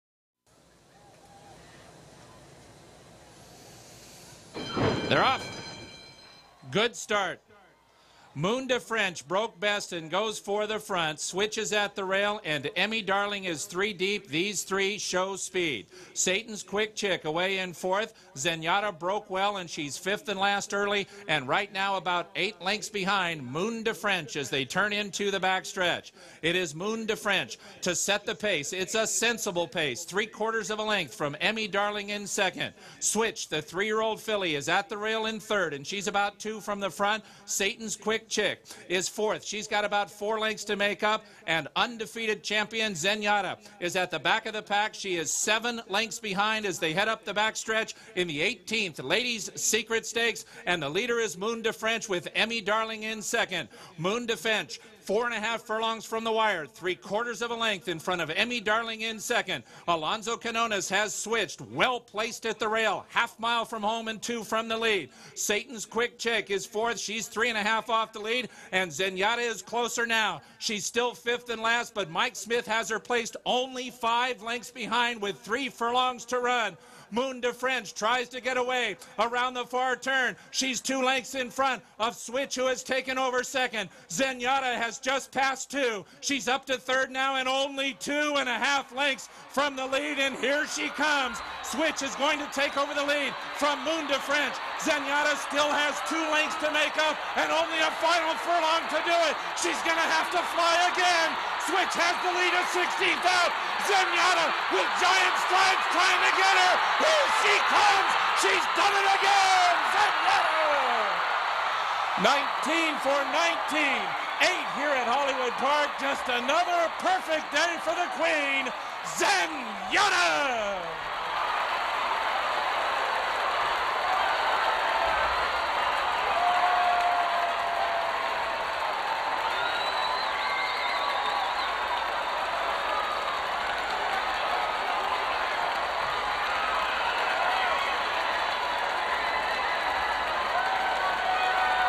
Zenyatta A Perfect 19 For 19 After Victory In Lady's Secret
Making her final start in Southern California at the place where she has been stabled throughout her brilliant career, Zenyatta remained undefeated with a thrilling victory in the $250,000 Lady's Secret Stakes Saturday at Oak Tree/Hollywood Park.

The race was one of four Grade I events on the card.

Before an extremely enthusiastic crowd of 25,837, the 6-year-old daughter of Street Cry and the Kris S. mare Vertigineux won for the 19th time -- matching the streak of Peppers Pride, who earned her wins in races restricted to New Mexico breds -- with a strong rally to defeat 4-1 second choice Switch by a half-length.

The victory was an unprecedented third in a row in the Lady's Secret for Zenyatta, who is owned by Jerry and Ann Moss and trained by John Shirreffs, and enabled her to become the leader in career earnings by a female thoroughbred.

The $150,000 payday pushed her bankroll to $6,404,580, surpassing Ouija Board, who retired in 2006 with earnings of $6,312,552.

Winning for the eighth time at Hollywood Park, Zenyatta, who trailed for more than six furlongs under jockey Mike Smith, completed the 1 1/16 miles over Cushion Track in 1:42.97. She ran the last sixteenth of a mile in less than six seconds.

The 1-10 favorite, Zenyatta returned $2.20 and $2.10. There was no show wagering.